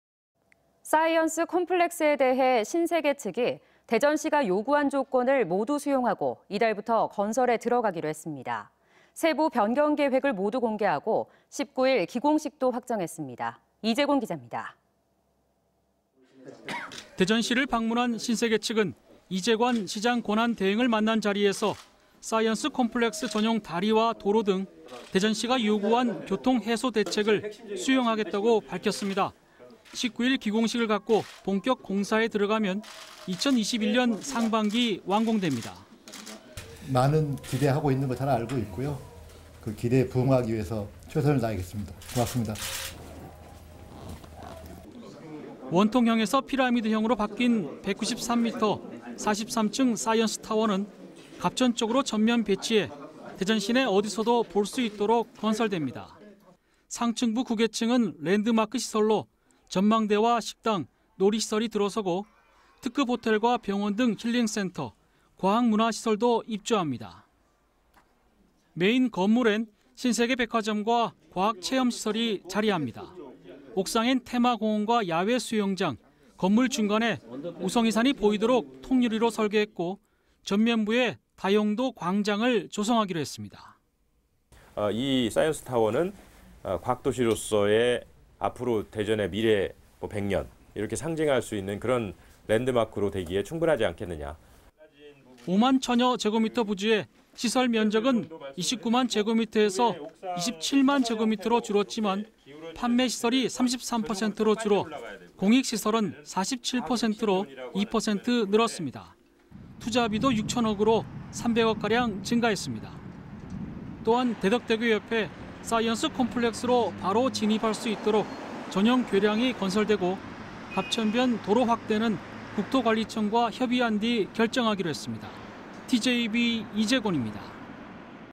[TJB뉴스]사이언스 콤플렉스 ..19일 첫 삽
【 앵커멘트 】

사이언스 콤플렉스에 대해
신세계측이
대전시가 요구한 조건을
모두 수용하고 이달부터
건설에 들어가기로 했습니다.

세부 변경계획을
모두 공개하고,
19일 기공식도
확정했습니다.

이재곤 기자.